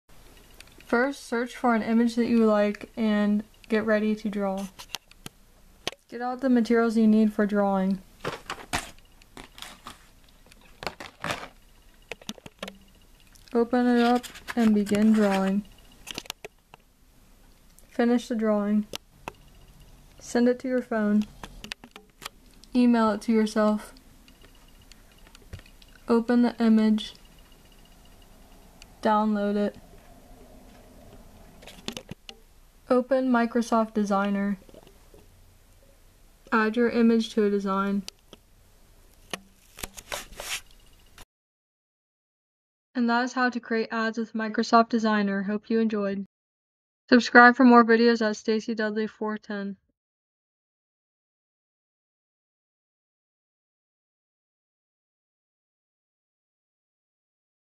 Microsoft Designer Tutorial
Learn how you can do marketing by first creating a design and then uploading it to Microsoft Designer for free! Then use their templates to create your own ads, posters, fliers and videos!